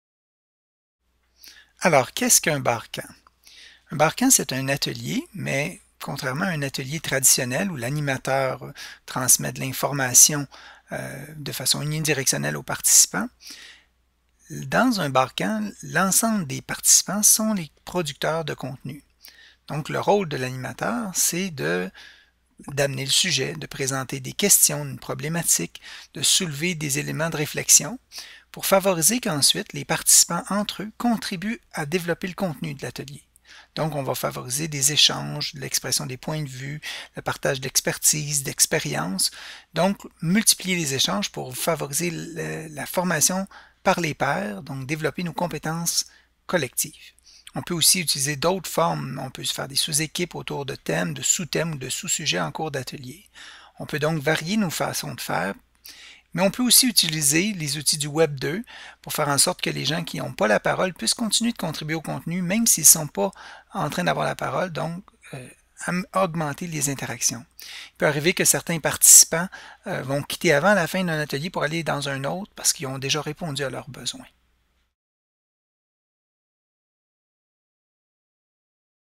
Qu'est-ce qu'un BarCamp?
Brève description de qu'est un BarCamp. Pour plus de détails, voir la définition sur Wikipedia.
Réalisation, montage et narration par Benoit Petit